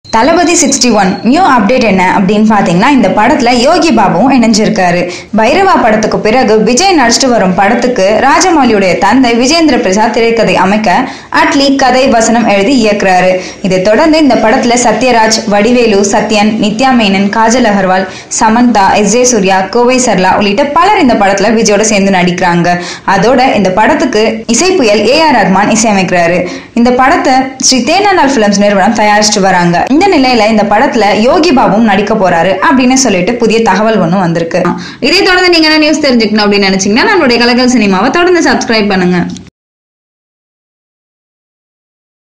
Another Comedy actor joins in Thalapathy 61
அஜித்தை தொடர்ந்து விஜய்யுடன் இணைந்த மற்றொரு காமெடி நடிகர்!